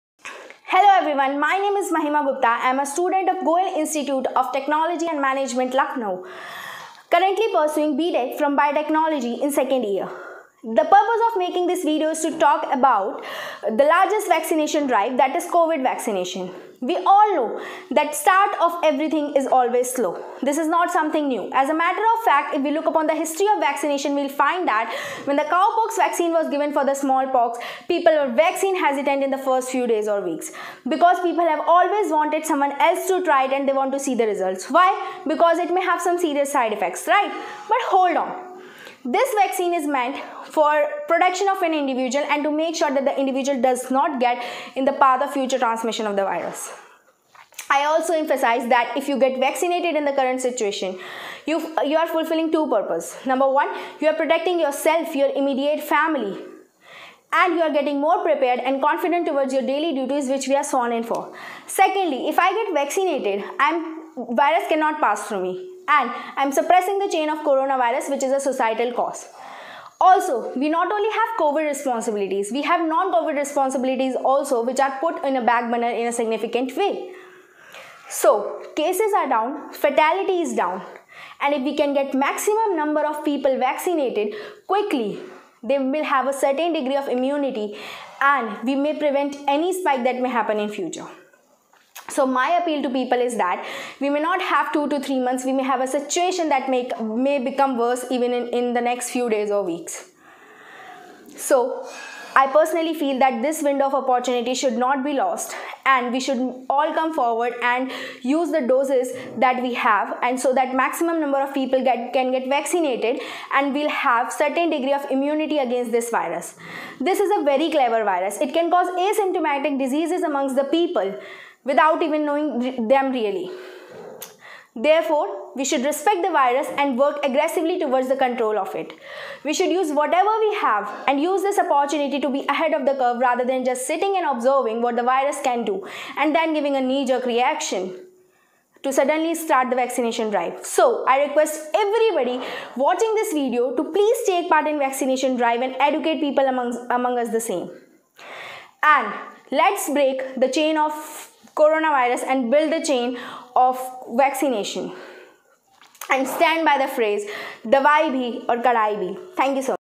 Mahima Gupta motivating for Vaccination
Mahima Gupta Student of 2nd year Biotech giving a motivational video on vaccination for Corona virus